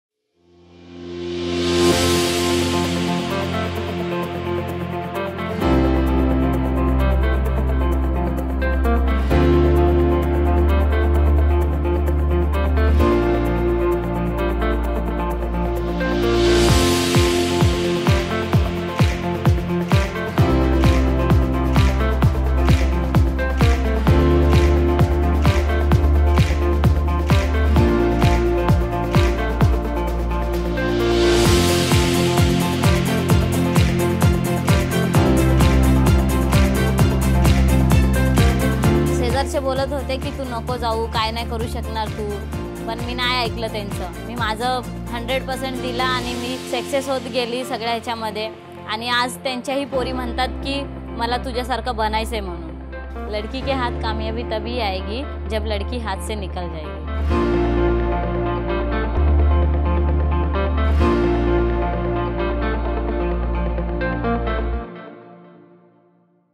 Arati Patole | LadkiHaathSeNikalJaayegi | Empowering the Girl Child | Mahindra Rise
A national level award & representing India on the International stage, Arati Patole is changing the meaning of LadkiHaathSeNikalJaayegi one achievement at a time. Check out her story!

To know what else we do to serve the community, go